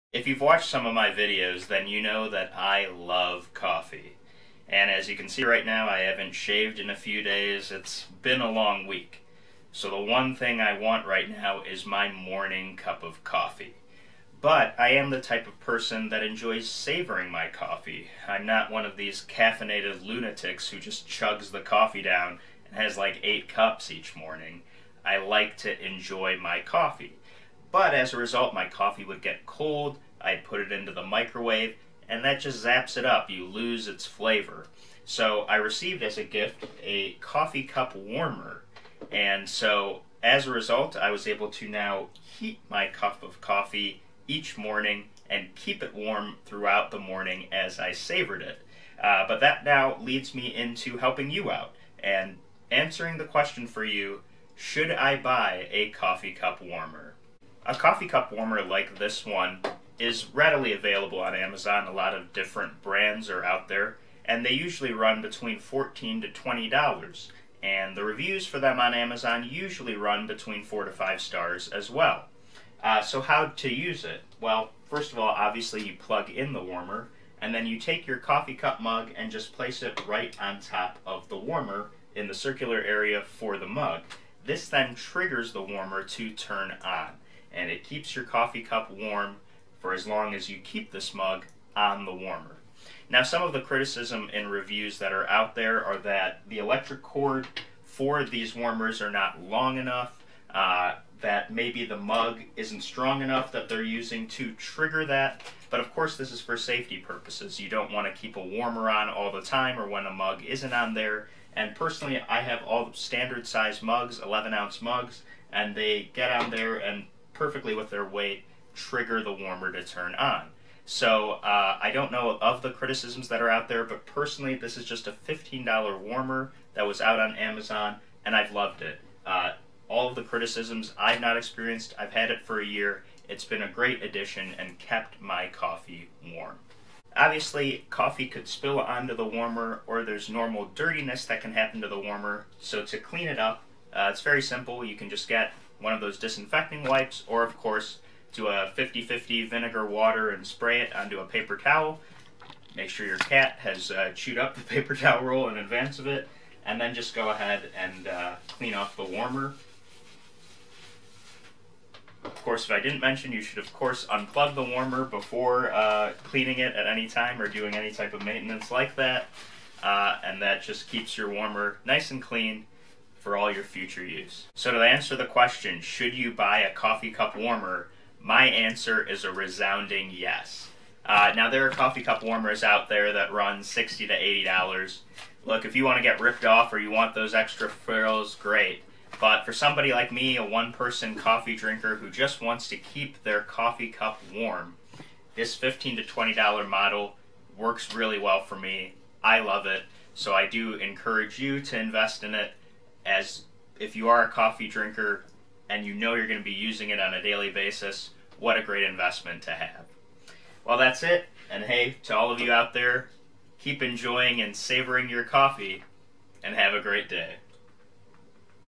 Should I Buy a Coffee Cup Warmer?
If you enjoy savoring your coffee, Sumukh shares with you why investing in a coffee cup warmer is a worthwhile purchase!

Follow Sumukh on IG and Twitter - @sumukhcomedy